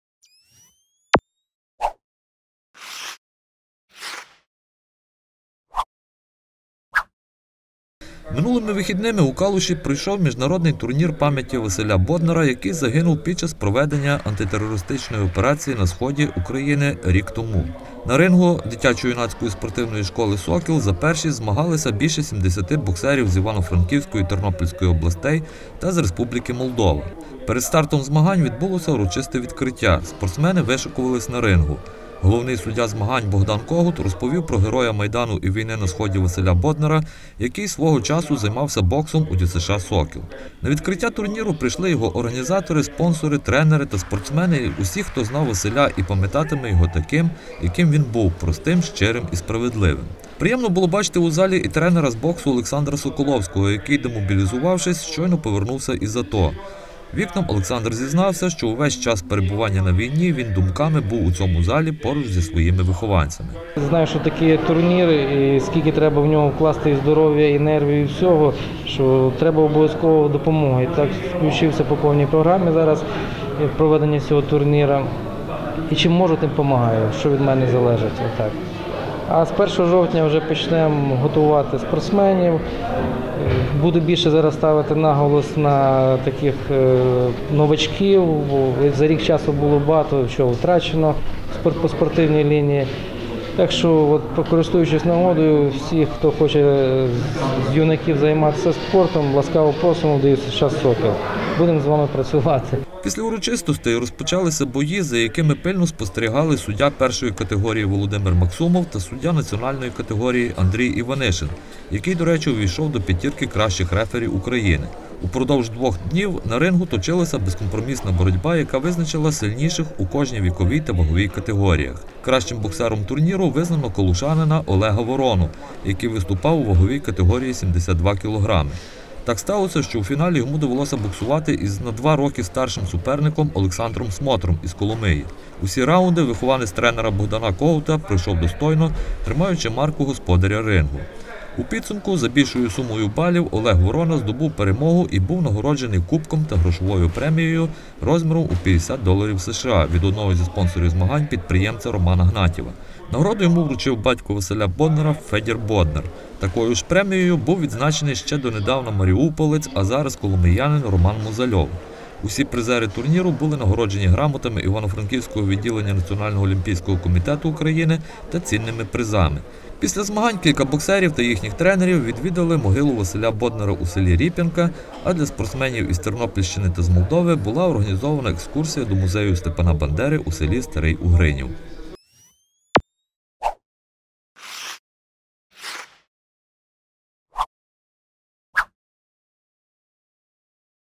Одним із переможців Міжнародного турніру пам’яті героя АТО став маріуполець з Коломиї
У Калуші пройшов міжнародний турнір пам’яті Василя Боднара, який загинув під час проведення антитерористичної операції на сході України рік тому. Детальніше за посиланням
Відео: Ігор Мудрик, Наталія Курдюкова, ВікнаТВ